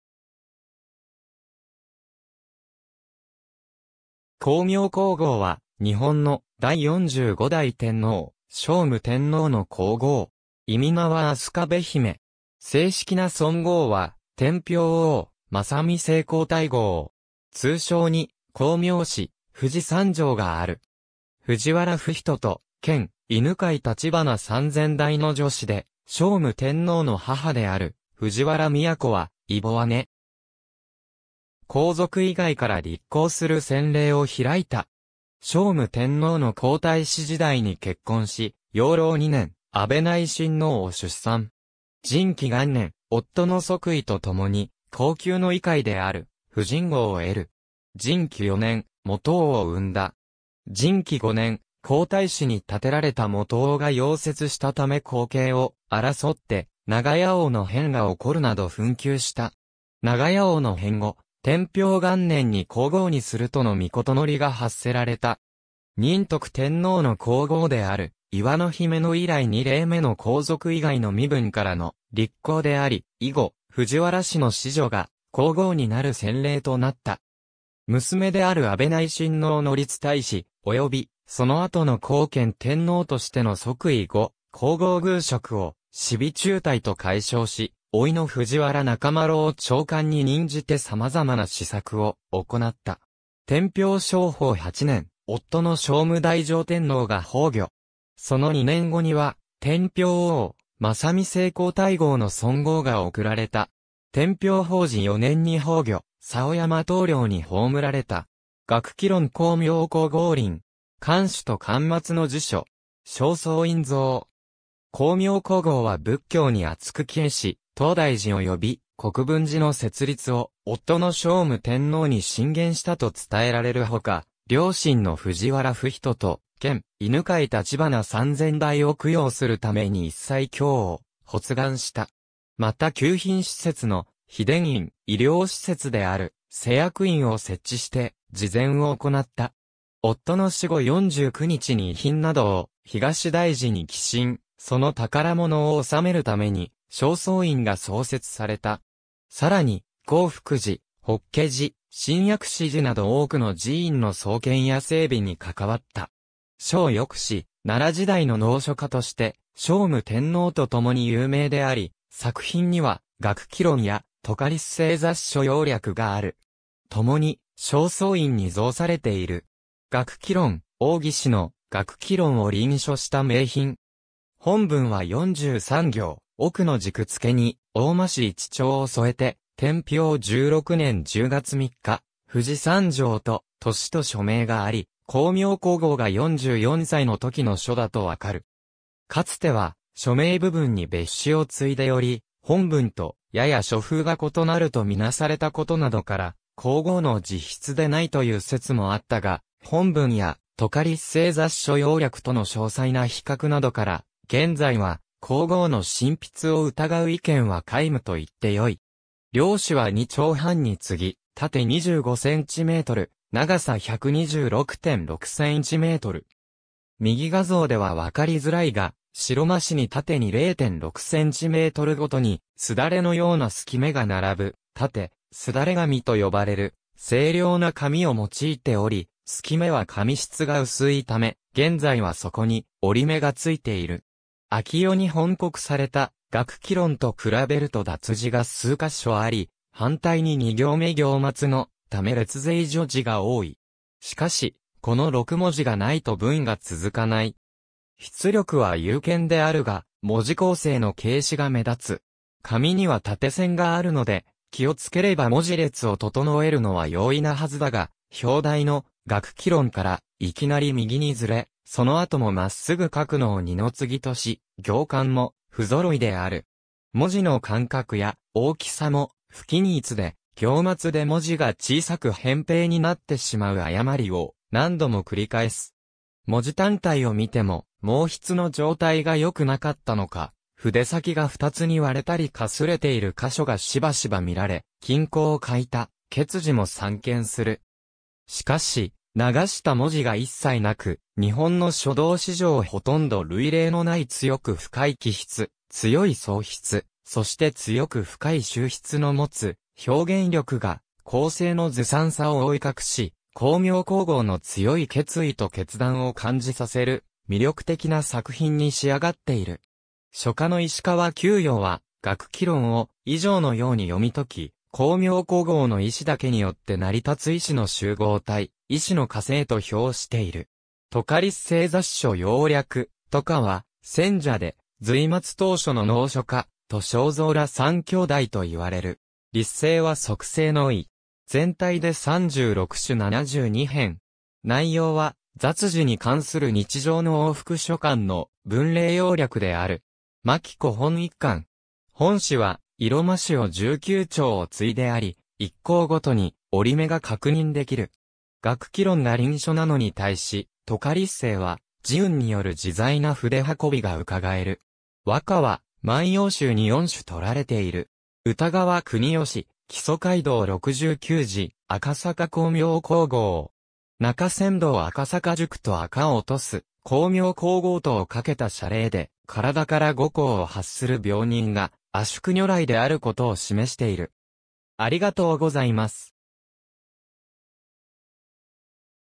光明皇后
光明皇后（こうみょうこうごう、701年〈大宝元年〉- 760年7月23日〈天平宝字4年6月7日〉）は、日本の第45代天皇・聖武天皇の皇后。
諱は安宿媛（あすかべひめ）。
正式な尊号は天平応真仁正皇太后（てんぴょうおうしんにんしょうこうたいごう）。
通称に光明子（こうみょうし）、藤三娘（とうさんじょう）がある。
藤原不比等と県犬養橘三千代の女子で、聖武天皇の母である藤原宮子は異母姉。
皇族以外から立后する先例を開いた。
聖武天皇の皇太子時代に結婚し、養老2年（718年）、阿倍内親王（後の孝謙・称徳天皇（重祚））を出産。
神亀元年（724年）、夫の即位とともに後宮の位階である夫人号を得る。
神亀4年（727年）、基王を生んだ。
神亀5年（728年）、皇太子に立てられた基王が夭折したため後継を争って長屋王の変が起こるなど紛糾した。
長屋王の変後、天平元年（729年）に皇后にするとの詔が発せられた。
仁徳天皇の皇后である磐之媛以来2例目の皇族以外の身分からの立后であり、以後、藤原氏の子女が皇后になる先例となった。
娘である阿倍内親王の立太子、および、その後の孝謙天皇としての即位（天平勝宝元年（749年））後、皇后宮職を紫微中台と改称し、甥の藤原仲麻呂を長官に任じて様々な施策を行った。
天平勝宝8年（756年）、夫の聖武太上天皇が崩御。
その2年後には「天平応真仁正皇太后」の尊号が贈られた。
天平宝字4年（760年）に崩御、佐保山東陵に葬られた。
楽毅論 光明皇后臨。
巻首（右）と巻末（左）の自署。
正倉院蔵。
光明皇后は仏教に篤く帰依し、東大寺および国分寺の設立を夫の聖武天皇に進言したと伝えられるほか、両親の藤原不比等と県犬養橘三千代を供養するために一切経を発願した（五月一日経）。
また救貧施設の「悲田院」、医療施設である「施薬院」を設置して慈善を行った。
夫の死後四十九日に遺品などを東大寺に寄進、その宝物を収めるために正倉院が創設された。
さらに、興福寺、法華寺、新薬師寺など多くの寺院の創建や整備に関わった。
書をよくし、奈良時代の能書家として聖武天皇とともに有名であり、作品には『楽毅論』（がっきろん）や『杜家立成雑書要略』（とかりっせいざっしょようりゃく）がある。
ともに正倉院に蔵されている。
楽毅論 王羲之の『楽毅論』を臨書した名品。
本文は43行、奥の軸付に黄麻紙一帳を添えて「天平十六年十月三日（744年11月11日）藤三娘」と、年紀と署名があり、光明皇后が44歳の時の書だと分かる。
かつては署名部分に別紙を継いでおり、本文とやや書風が異なると見なされた事などから皇后の自筆でないという説もあったが、本文や『杜家立成雑書要略』との詳細な比較などから、現在は皇后の真筆を疑う意見は皆無と言って良い。
料紙は二帳半に継ぎ、縦25cm、長さ126.6cm。
右画像では分かりづらいが、白麻紙に縦に0.6cmごとに簾のような漉き目が並ぶ「縦簾紙」と呼ばれる精良な紙を用いており、漉き目は紙質が薄いため、現在はそこに折り目が付いている。
明代に翻刻された『楽毅論』と比べると脱字が数ヶ所あり、反対に2行目行末の「為劣是以叙而」が多い。
しかし、この6文字がないと文が続かない。
「筆力は雄健であるが、文字構成の軽視が目立つ。紙には縦線があるので気をつければ文字列を整えるのは容易なはずだが、表題の「楽毅論」からいきなり右にずれ、その後も真っ直ぐ書くのを二の次とし、行間も不揃いである。
文字の間隔や大きさも不均一で、行末で文字が小さく扁平になってしまう誤りを何度も繰り返す。
文字単体を見ても、毛筆の状態が良くなかったのか、筆先が2つに割れたりかすれている箇所がしばしば見られ、均衡を欠いた結字も散見する。
しかし、流した文字が一切なく、日本の書道史上殆ど類例のない強く深い起筆、強い送筆、そして強く深い終筆のもつ表現力が、構成の杜撰さを覆い隠し、光明皇后の強い決意と決断を感じさせる魅力的な作品に仕上がっている。
」書家の石川九楊は『楽毅論』を以上のように読み解き、光明皇后の意志だけによって成り立つ意志の集合体、「意志の化成」と評している。
杜家立成雑書要略 「杜家」は撰者で隋末唐初の能書家、杜正蔵ら三兄弟と言われる。
立成は速成の意。
全体で36種72篇。
内容は、雑事に関する日常の往復書簡の文例要略である。
巻子本1巻。
本紙は色麻紙を19張...